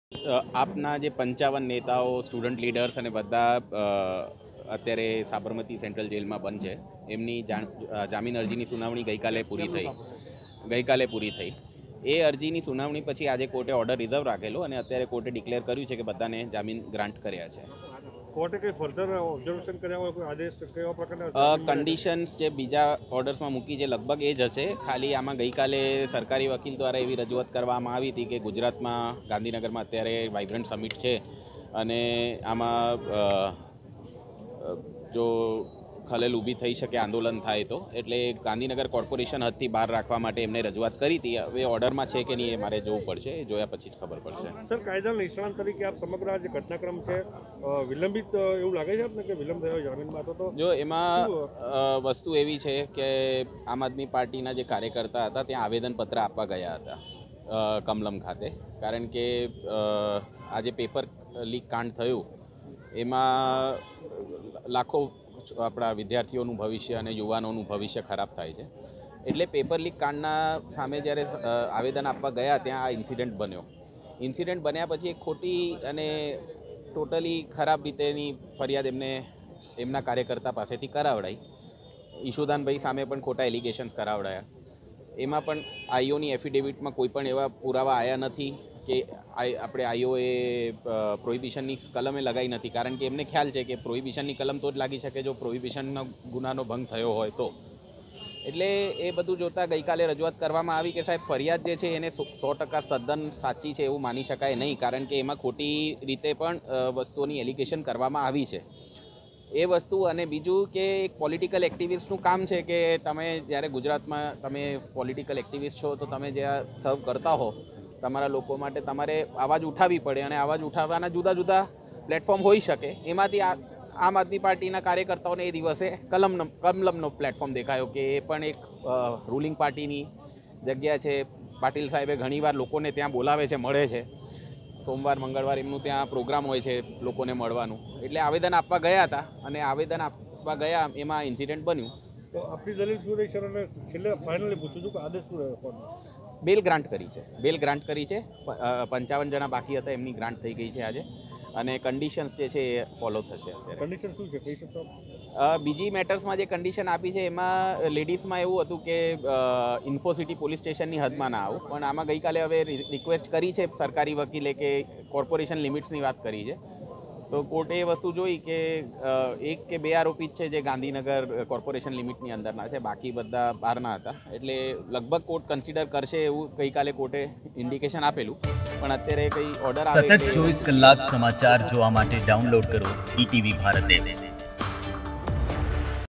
AAP protest at Kamalam: ગાંધીનગર કોર્ટે આપ પાર્ટીના 55 નેતા-કાર્યકર્તાઓને શરતી જામીન આપ્યા
કમલમના વિરોધ પ્રદર્શન (AAP protest at Kamalam) મામલે આપના નેતા અને કાર્યકર્તાઓને શરતી જામીન મળ્યા છે. કમલમ વિરોધ પ્રદર્શનના કેસમાં મહિલાઓ સહિત આપના નેતા અને કાર્યકર્તાઓની ધરપકડ કરવામાં આવી હતી, પરંતુ ગાંધીનગર સેશન કોર્ટમાં 55ને શરતી જામીન આપવામાં આવ્યા છે. ત્યારે આજે સાંજે તમામને સાબરમતી જેલમાંથી છોડવામાં આવે તેવી શકયતા છે.